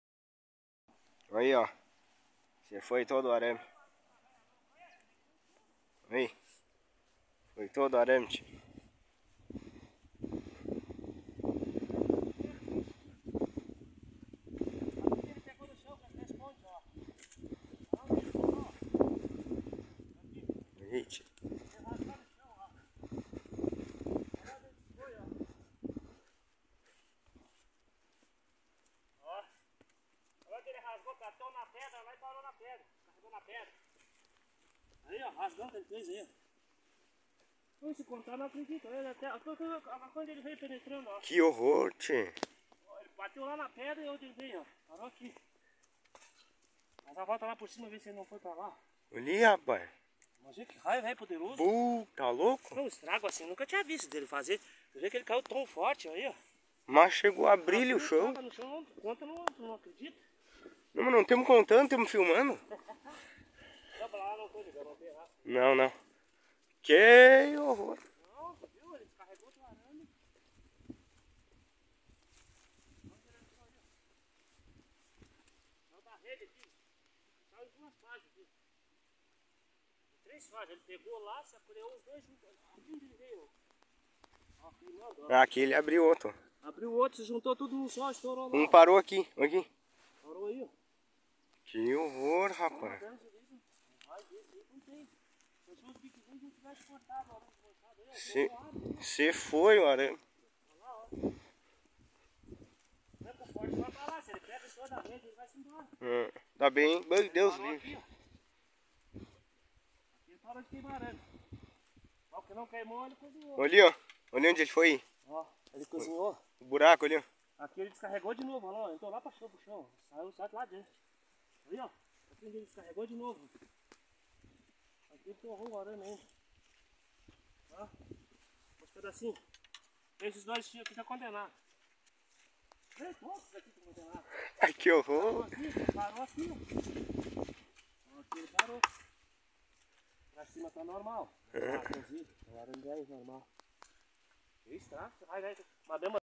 Raio atingiu propriedade rural no 3° distrito de Canguçu
Vídeo de WhatsApp.  fato entre terça e quarta feira, 18 e 19 de Junho 2024